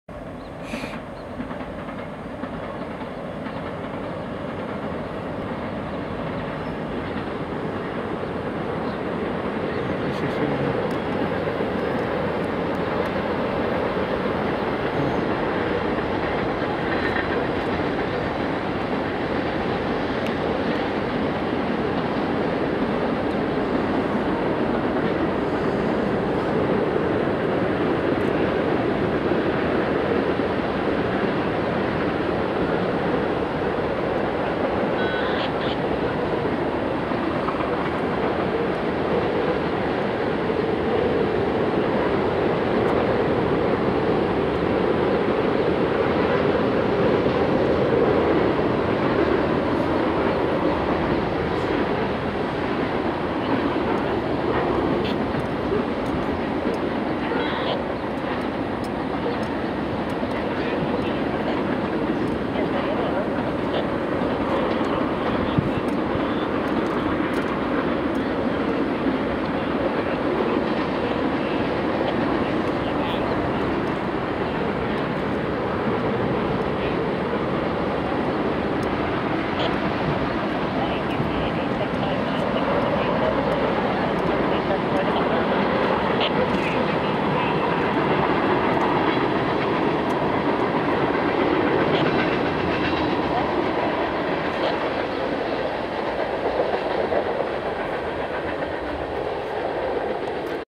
UP 8659 LEADS MSSPR EAST THRU SUSSEX, (THE SURPRISE IN SUSSEX) 6-4-11.MOD
UP 8659 LEADS MSSPR EAST THRU SUSSEX, EN ROUTE TO BUTLER. I GOT THIS TRAIN IN SUSSEX WHILE WAITING FOR TRAIN 347 ON THE CN.